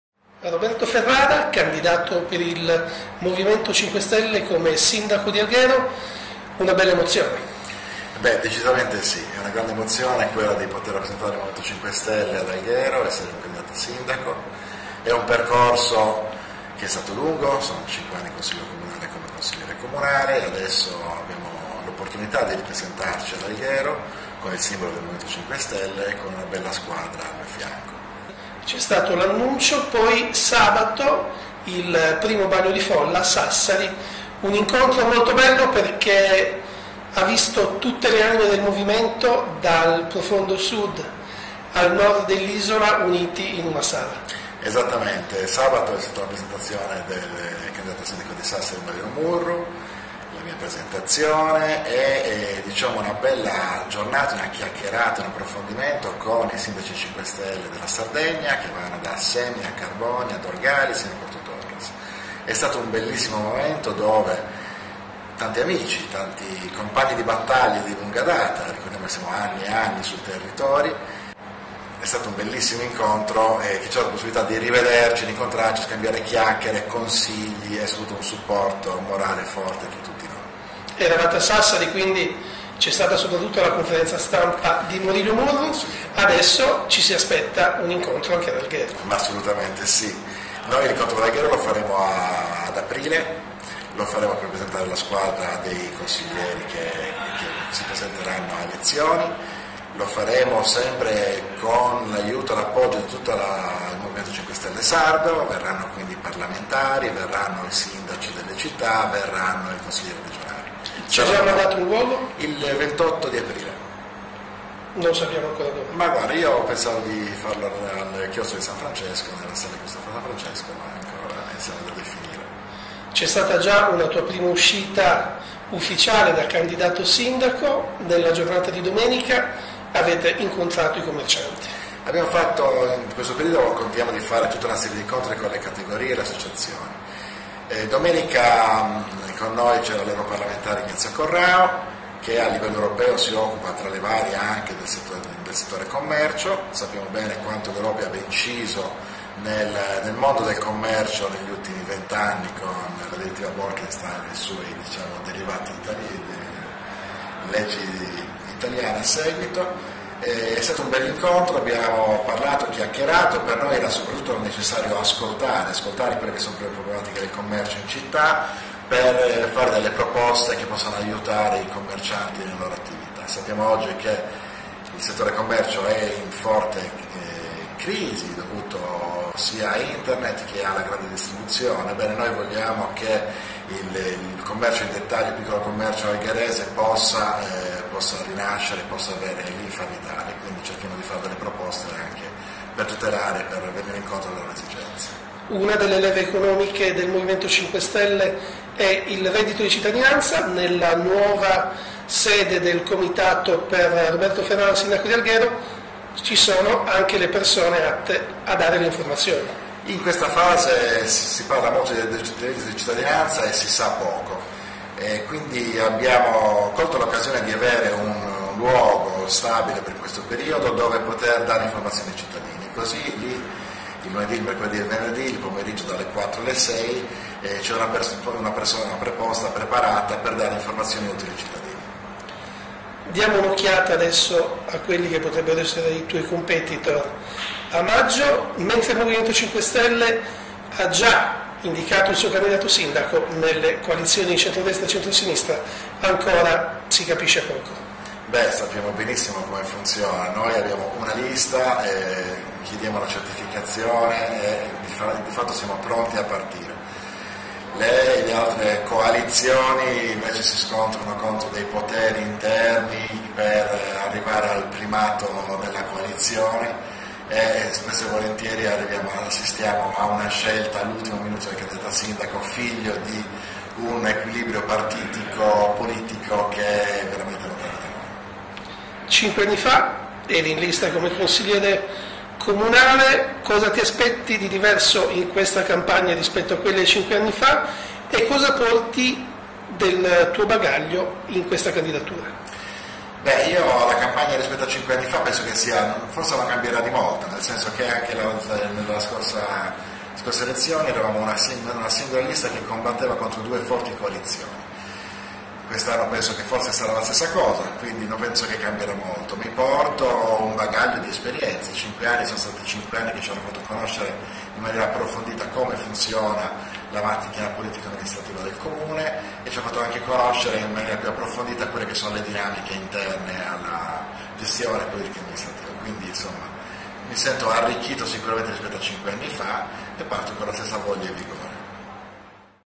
Roberto Ferrara è il candidato a 5 Stelle
Roberto Ferrara, candidato sindaco nella città di Alghero in occasione delle elezioni amministrative in Sardegna, traccia la linea del partito fondato da Beppe Grillo. Emozione e decisione, un mix vincente su cui fondare il grande sogno di vedere la cittadina a nord ovest dell'isola a guida Pentastellata.